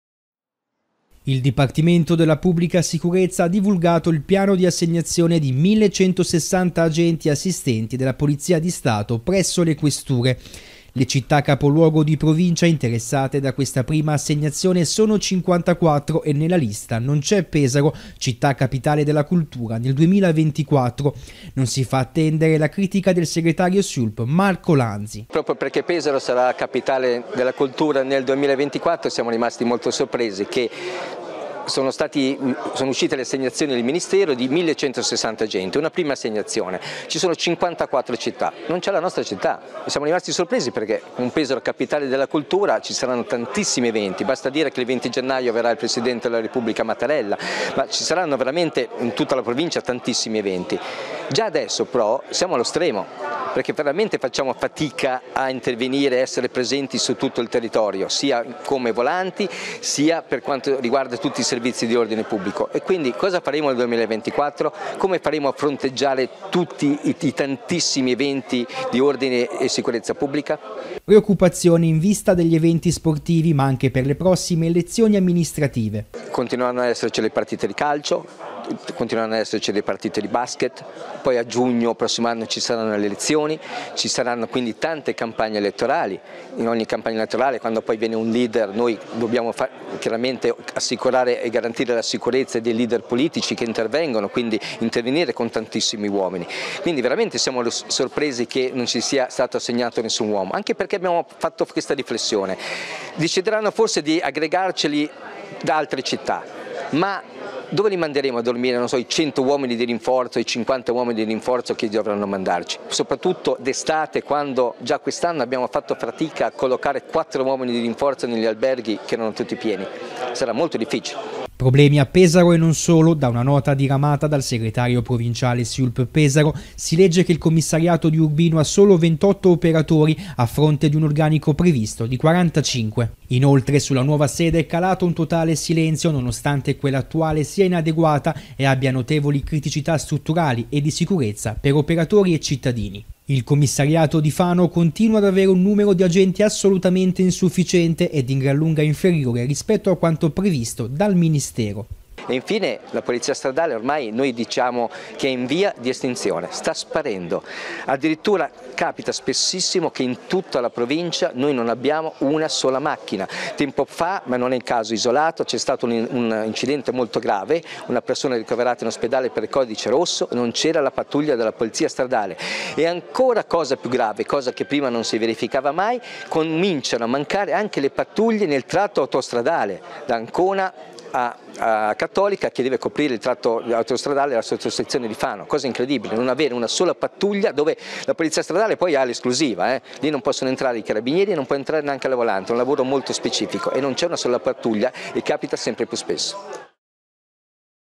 Forze dell'ordine allo stremo: l'appello del segretario Siulp
A Pesaro, città Capitale della Cultura 2024, nessun rinforzo per gli organici degli agenti di polizia. Da qui nasce l'appello del segretario provinciale Siulp, Marco Lanzi che abbiamo intervistato ieri mattina.